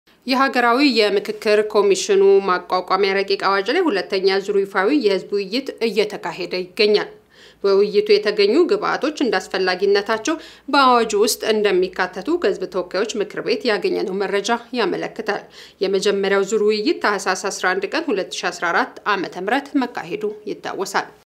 የሀገራዊ ምክክር ኮሚሽን
የሀገራዊ ምክክር ኮሚሽን ማቋቋሚያ ረቂቅ አዋጅ ላይ ሁለተኛው ዙር ይፋዊ የህዝብ ውይይት ተካሄደ፡፡